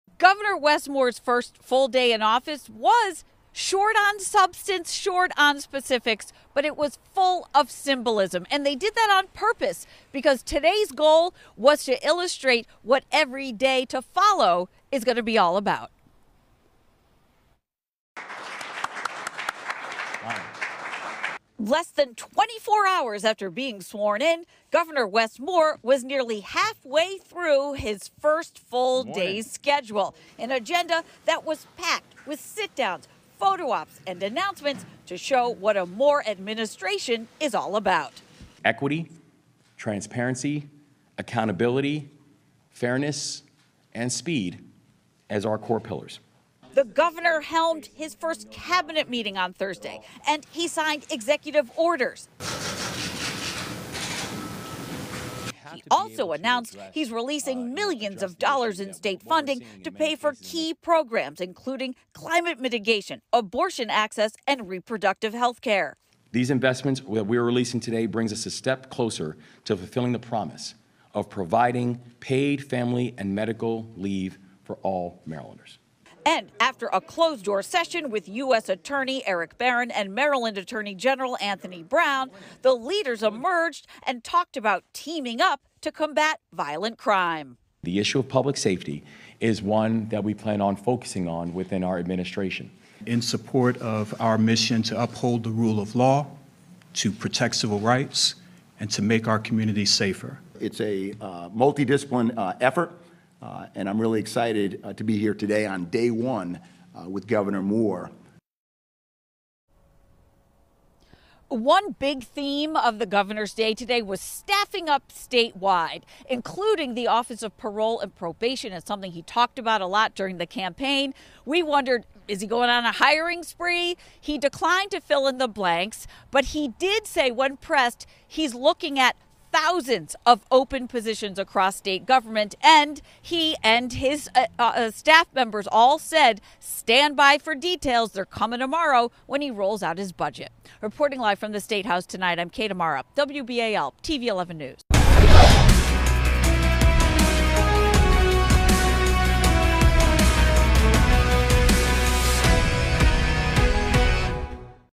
Gov. Moore releases $69M for key programs
Maryland Gov. Wes Moore got to work Thursday on his first full day in office, quickly addressing several policy issues. Less than 24 hours after he was sworn in, Maryland's 63rd governor was nearly halfway through an agenda packed with sit-down interviews, photo opportunities and announcements to show what a Moore administration is all about. The governor announced he's releasing $69 million in previously withheld state funding to pay for key programs. The funds were authorized by the General Assembly last year, according to the governor's office, which said $9 million will support the Climate Solutions Now Act, $10 million will support paid family and medical leave, $46.5 million will support cannabis reform and $3.5 million will support access to abortion care.